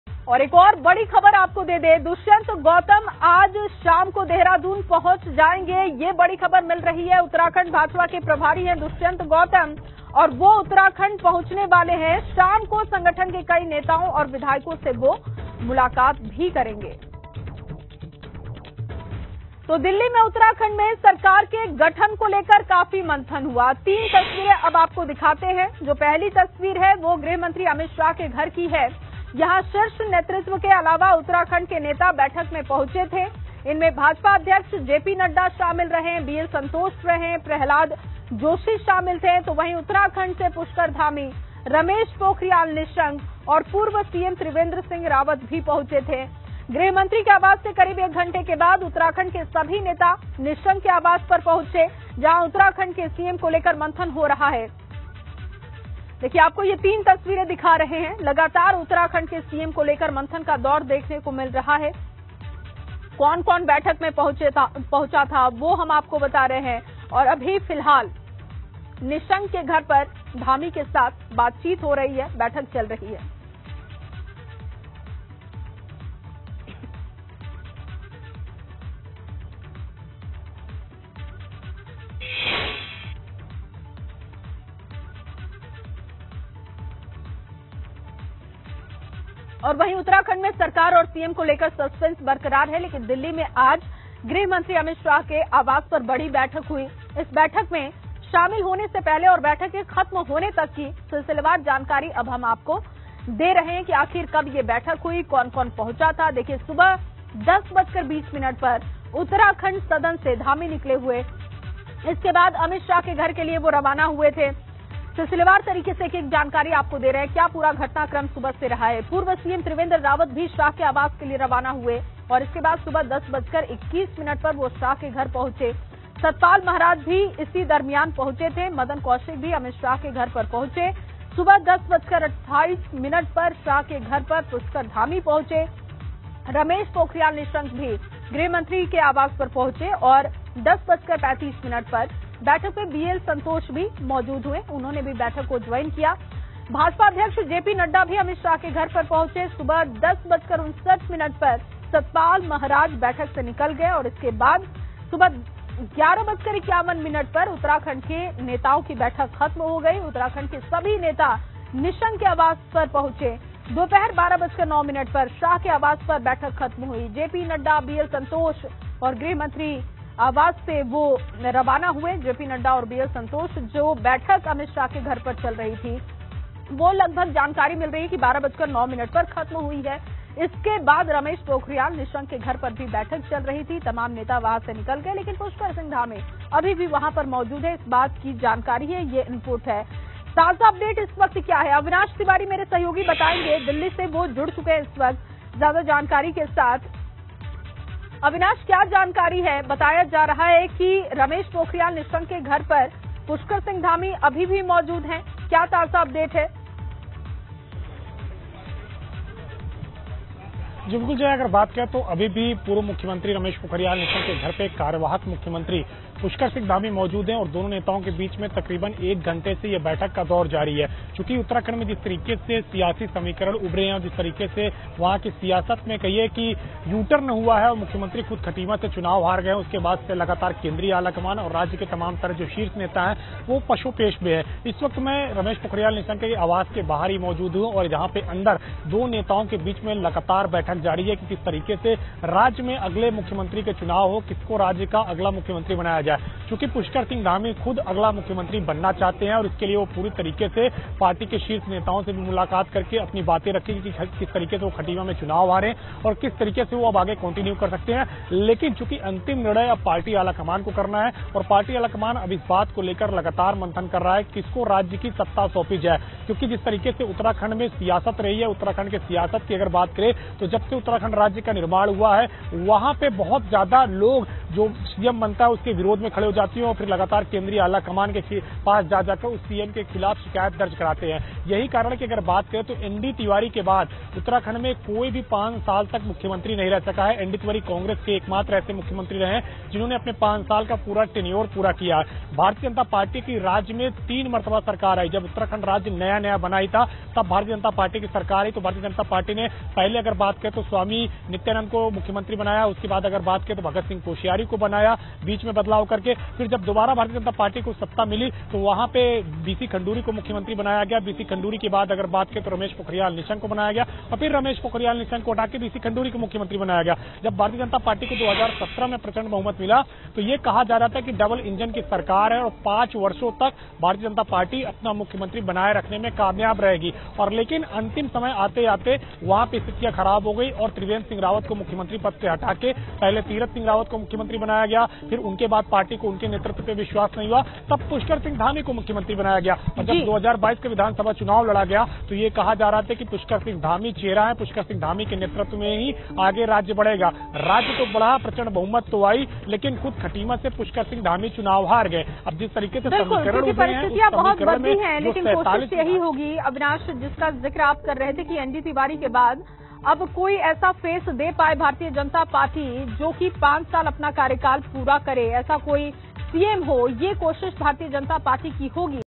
Uttarakhand BJP के प्रभारी Dushyant Gautam देहरादून पहुंच कई नेताओं से करेंगे मुलाकात
Uttarakhand BJP के प्रभारी Dushyant Gautam देहरादून पहुंच कई नेताओं से करेंगे मुलाकात, देखिए...